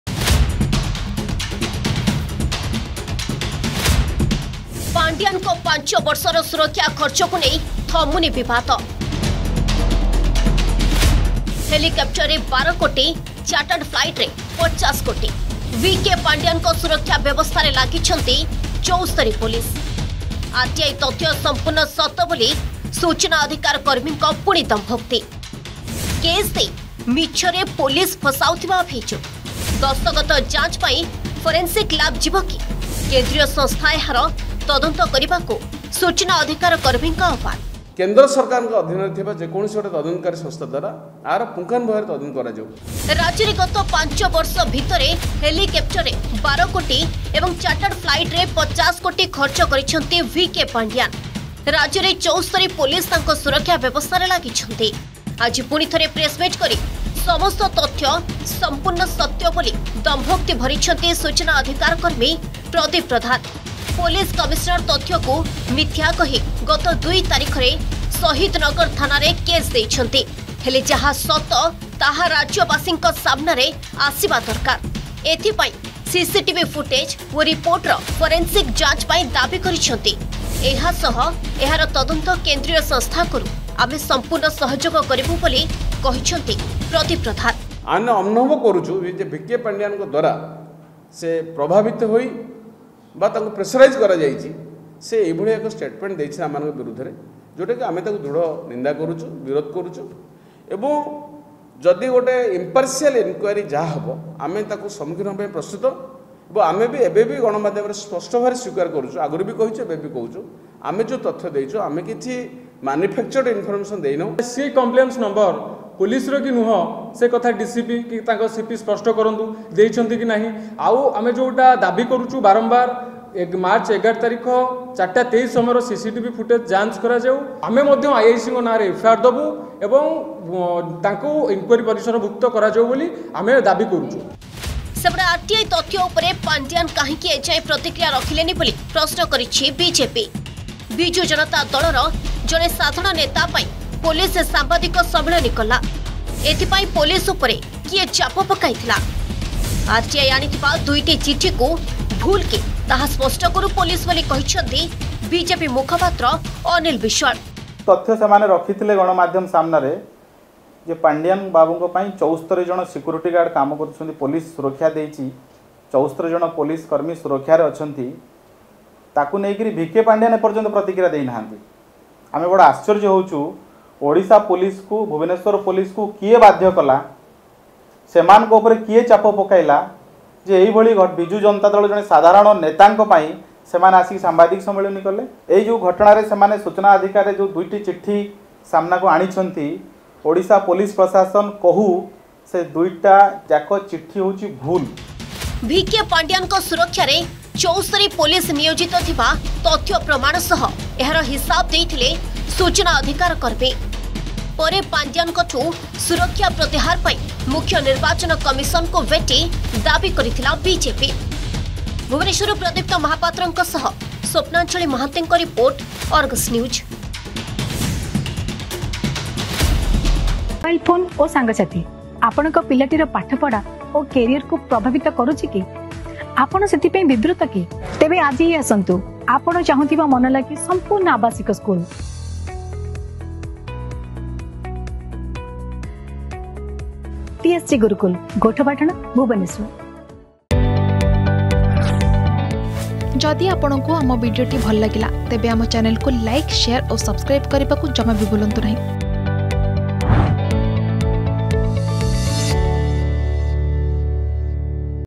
ଭି.କେ ପାଣ୍ଡିଆନଙ୍କ 5 ବର୍ଷର ସୁରକ୍ଷା ଖର୍ଚ୍ଚ ବିବାଦ || V.K Pandian
ଭି.କେ ପାଣ୍ଡିଆନଙ୍କ 5 ବର୍ଷର ସୁରକ୍ଷା ଖର୍ଚ୍ଚ ବିବାଦ । ହେଲିକପ୍ଟରରେ 12 କୋଟି, ଚାର୍ଟାଡ୍ ଫ୍ଲାଇଟରେ 50 କୋଟି ଖର୍ଚ୍ଚ କରିଛନ୍ତି ପାଣ୍ଡିଆନ । 5T ଅଧ୍ୟକ୍ଷଙ୍କ ସୁରକ୍ଷା ବ୍ୟବସ୍ଥାରେ ଲାଗିଛନ୍ତି 74 ପୋଲିସ୍ । RTI ତଥ୍ୟ ସମ୍ପୂର୍ଣ୍ଣ ସତ ବୋଲି ପ୍ରେସମିଟ୍ କରି ପୁଣି ଦମ୍ଭୋକ୍ତି ଭରିଛନ୍ତି ସୂଚନା ଅଧିକାର କର୍ମୀ ।